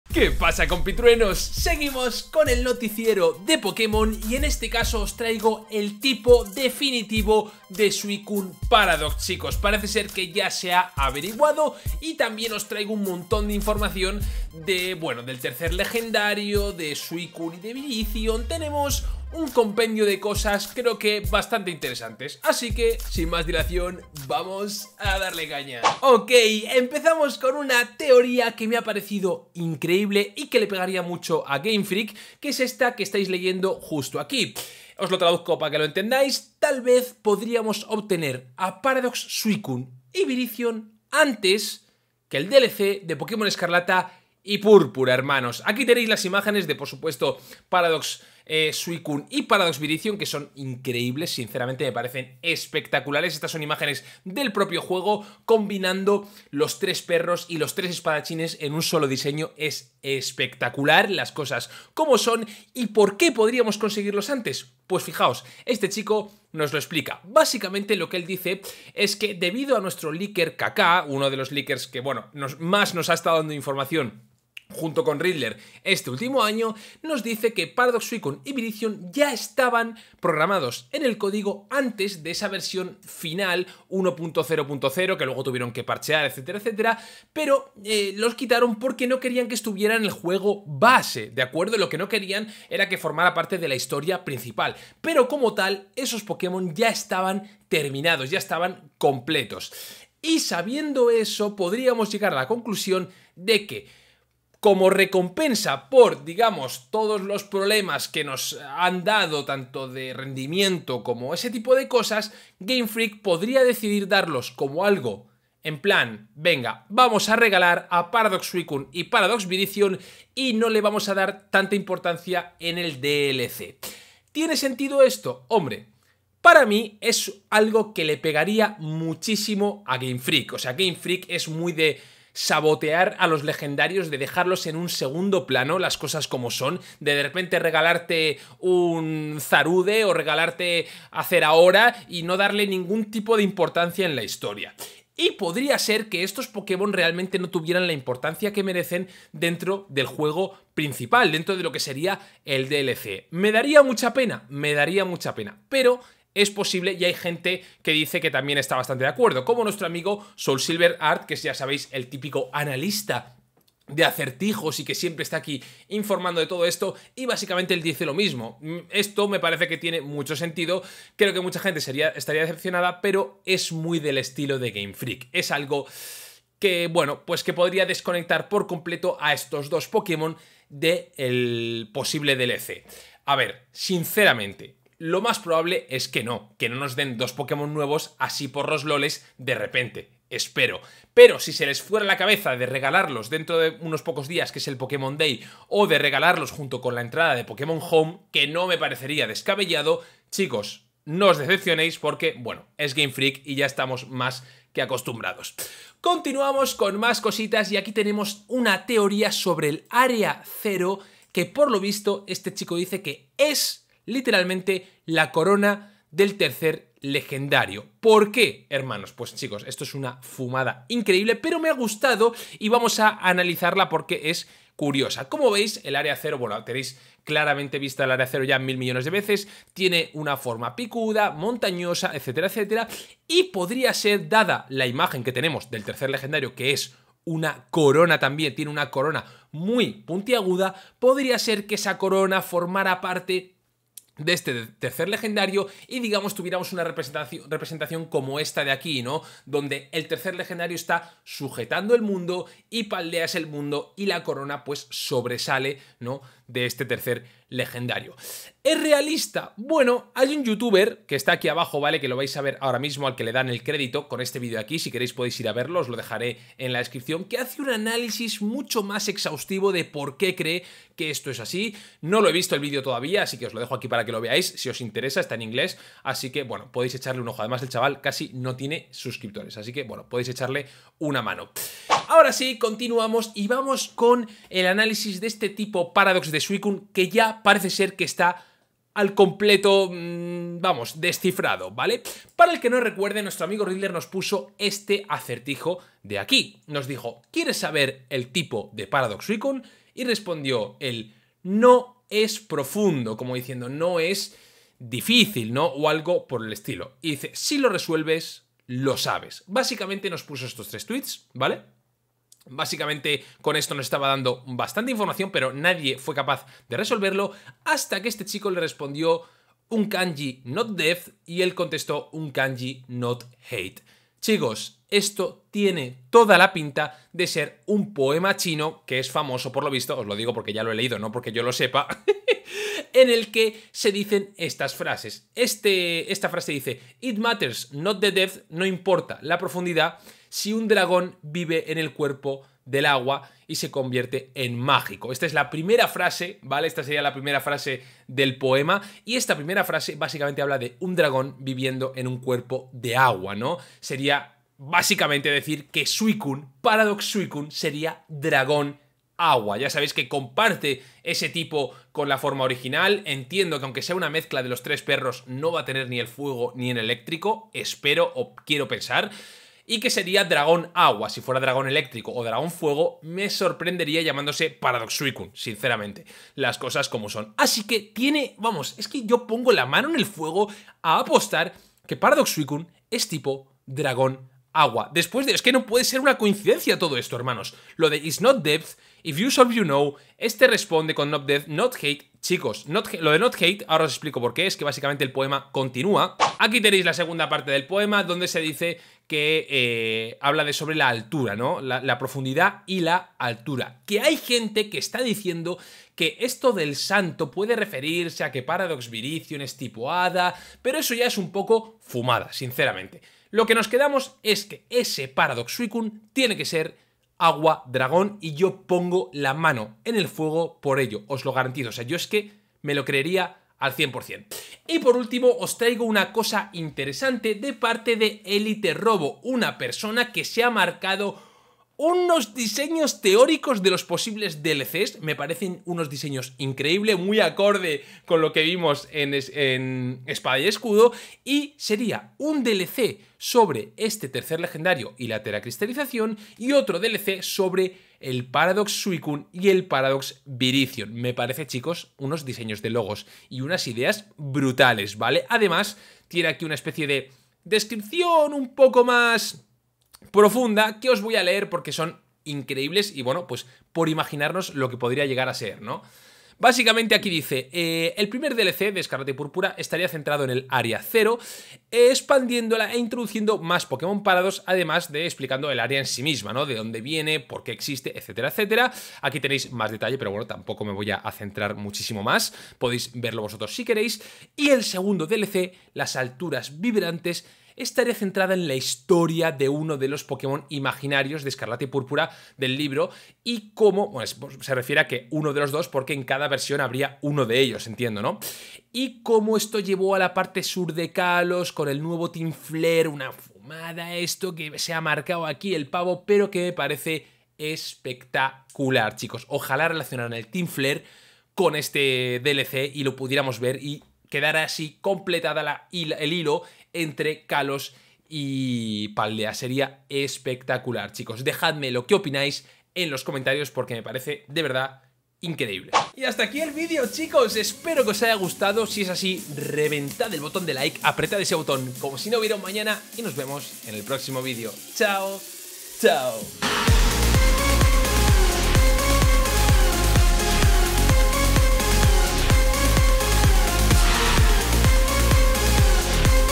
¡¡SUICUNE PARADOX es TIPO DRAGÓN!! 🐉 ¿POSIBLES TÍTULOS de DLCS? || Leaks Pokémon Escarlata y Púrpura
¡¡SE FILTRAN los TIPOS del DLC!! Ya hay GENTE FILTRANDO el DLC de Pokémon Escarlata y Pokémon Púrpura. TENEMOS MUCHOS MÁS LEAKS

🔽 ¡MIS REDES! 🔽

🔽 MARCADORES 🔽
0:00 Introducción SE FILTRA NUEVA INFO
0:25 PARADOX VIRIZION y SUICUNE ¿ANTES DEL DLC?
3:25 ÁREA ZERO es PARTE del TERCER LEGENDARIO
4:42 TIPOS de PARADOX SUICUNE
7:04 POSIBLES TÍTULOS para los DLCS
8:45 Suscríbete PERRO